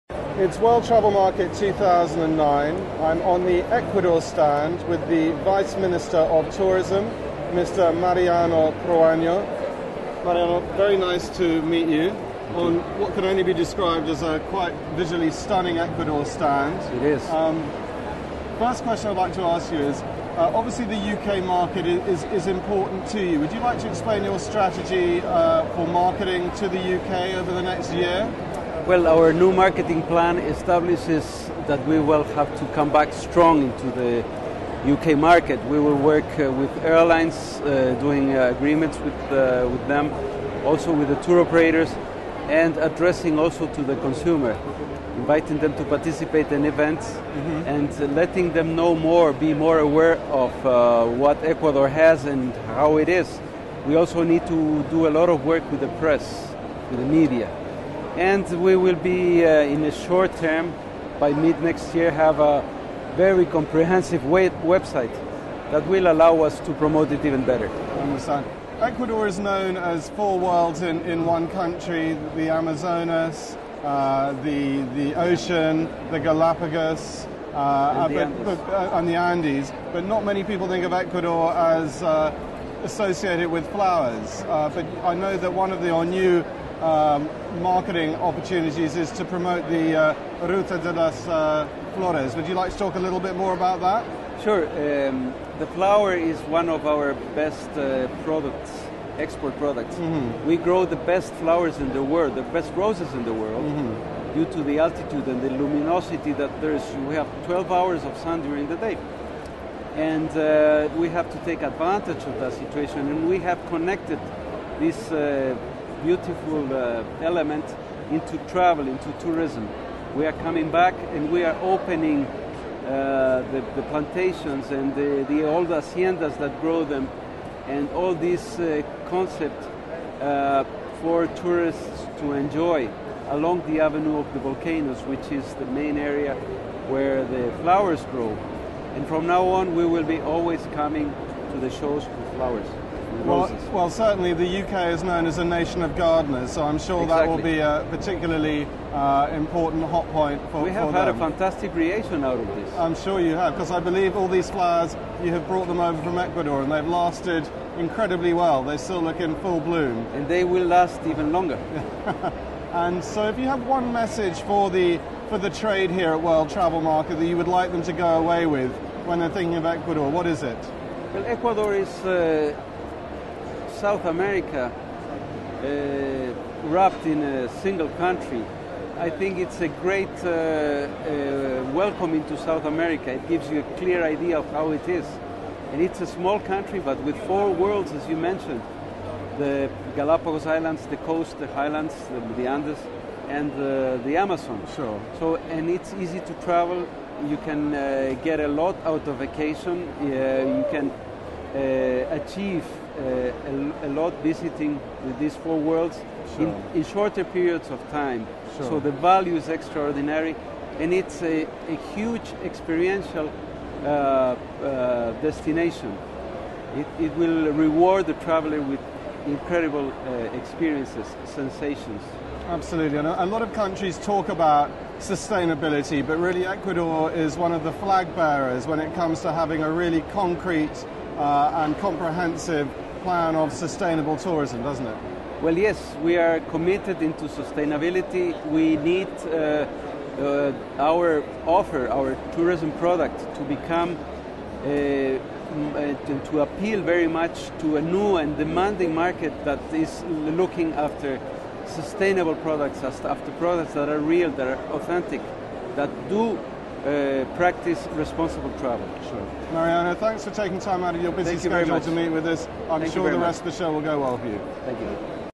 Ecuador Tourism
Interview with the Ecuador Tourism by Exposure TV  the World Travel Market 2009